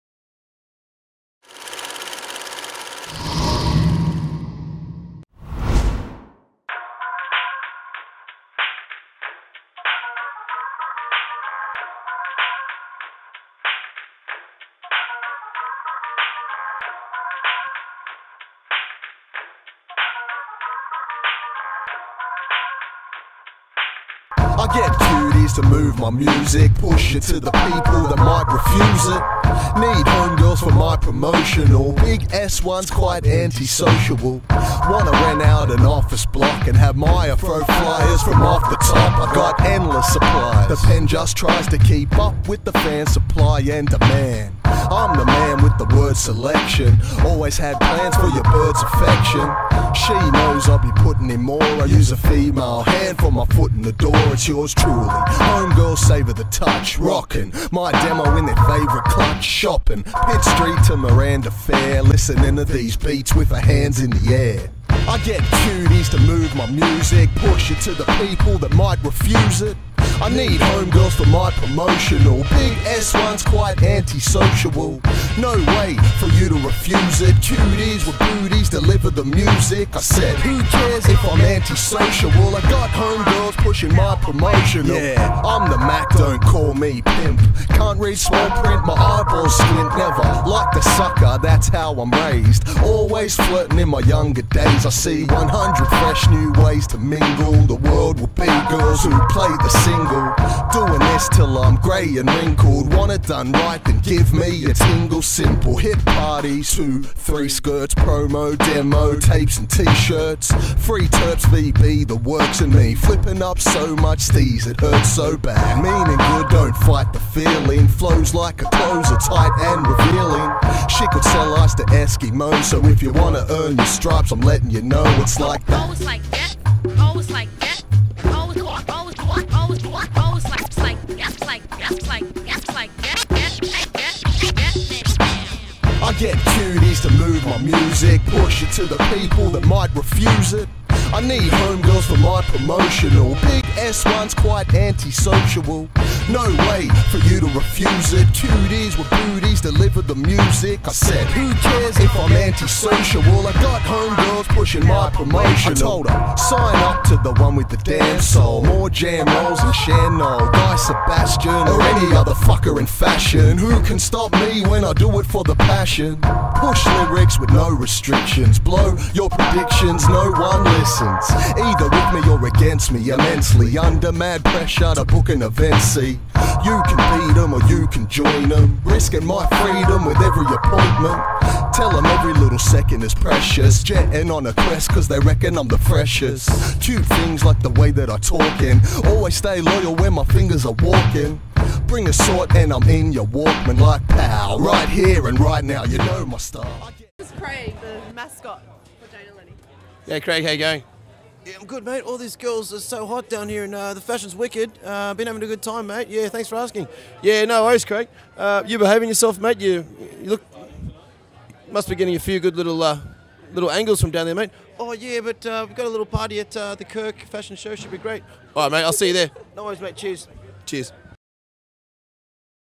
UnseenTV Jane & Eleni - RAFW
Sydney based fashion label jane & eleni is a collaboration between mother and daughter. Founded in 2007, jane&eleni has developed a unique style and a strong following.Playful silhouettes, floral shapes and a pastel palette.